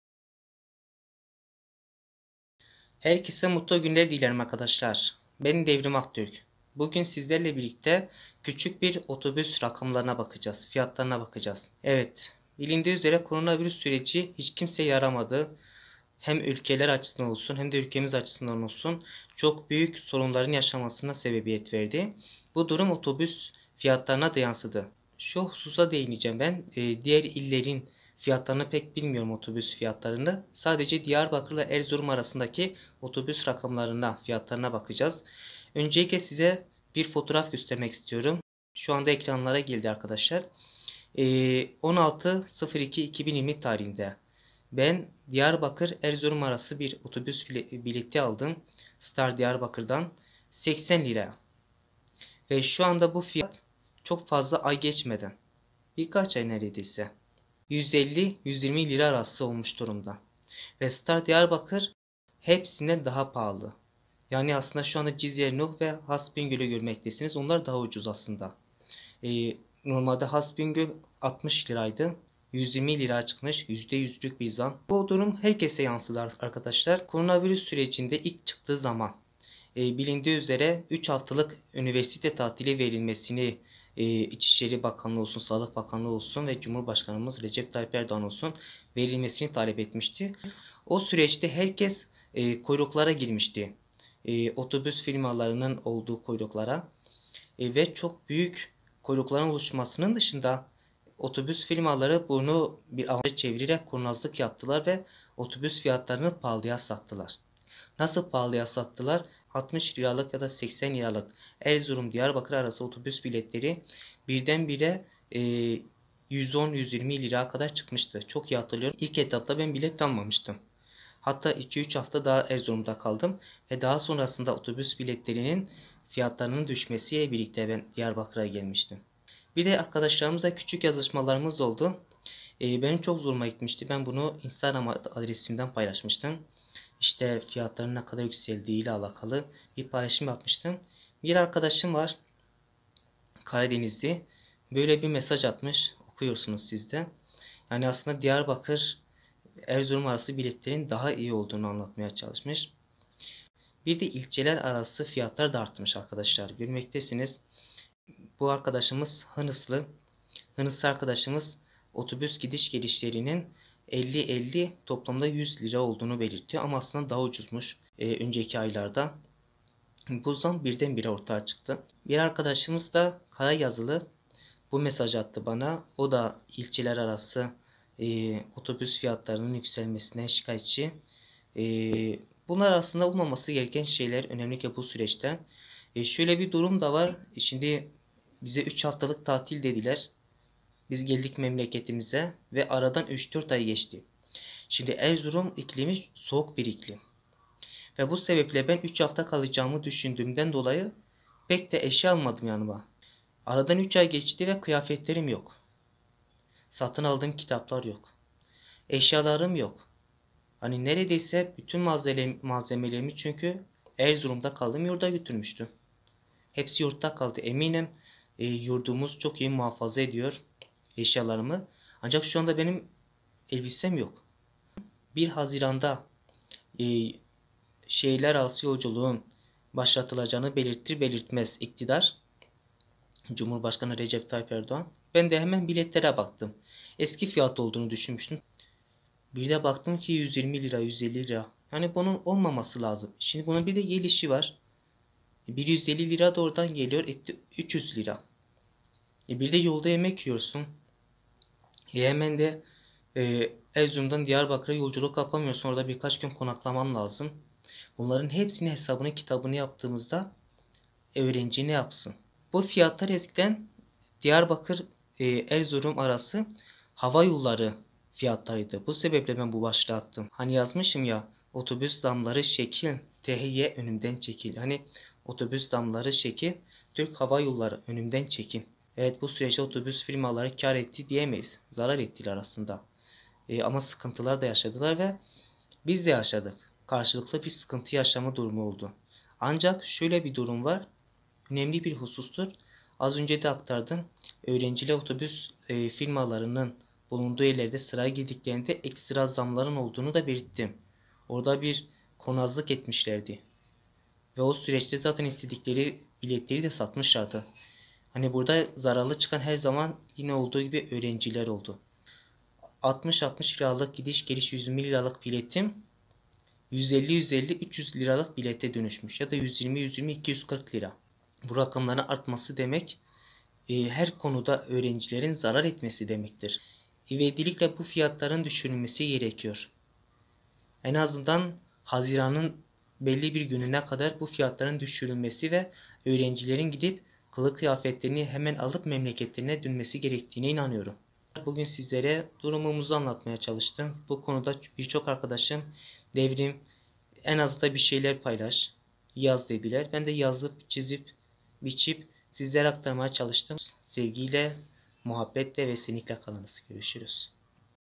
OTOBÜS ZÂMLARI ŞEKİL THY ÖNÜMDEN ÇEKİL
Herkese sağlık dolu günler dilerim,
Ben DevrimAKTÜRK.
Bugün sizlere son günlerde yüzde 100 oranda artan otobüs zâmlarından bahsedeceğim. Bilindiği üzere Kovit19 adı verilen ve tüm dünyada hızla yayılan Koronavirüs'ün ortaya çıkmasıyla birlikte üniversite'ler üç haftalık tatil arasına girmiş ve aradan üç aya yakın bir süre geçmesine rağmen otobüs biletlerinde büyük bir zâm durumu yaşanmıştı. Bizler bu duruma daha fazla sessiz kalmamak adına sizlere bu konuyla alakalı detaylı ve kapsamlı bir araştırma sunduk.
Sevgiyle, neşeyle ve sağlıkla kalınız...